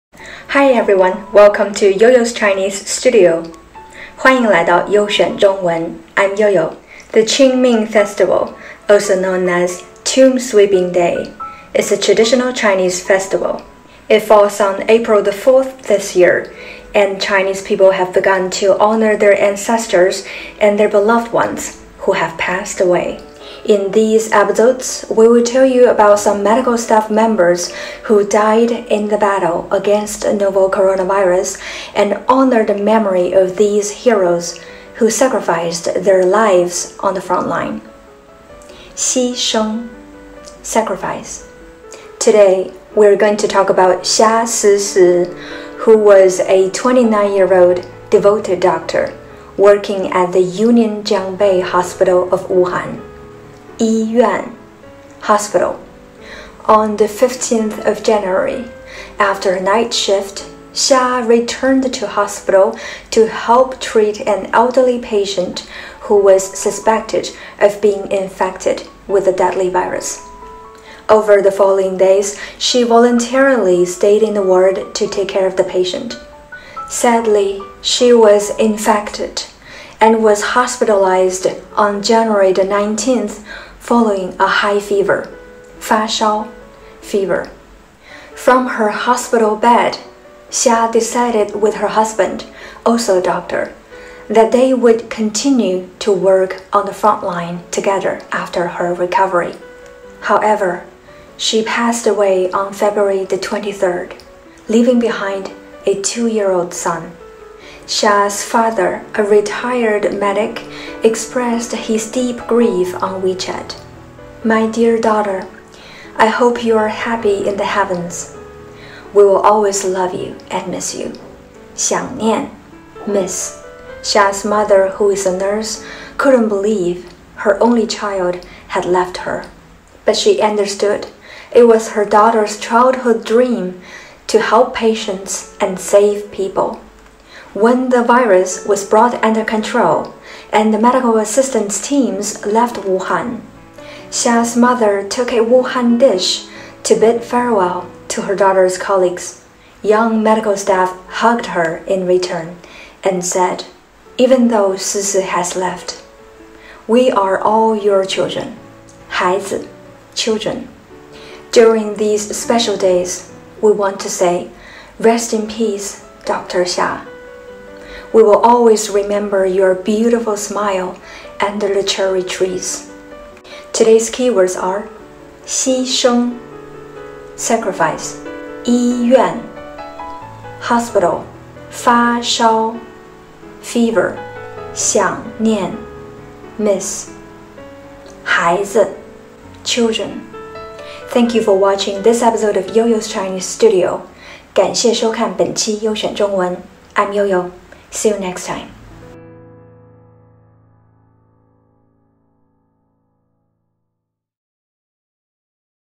Yo-Yo's Chinese Studio #5: Xia Sisi
【Yo-Yo’s Chinese Studio 悠选中文 清明节 致敬英雄系列 1 】
During this epidemic outbreak, many medical staff risk their lives and fight on the frontline. Some have even lost their lives. Yo Yo Wu brings you a story about a 29-year-old devoted doctor who sacrificed her life caring for the sick.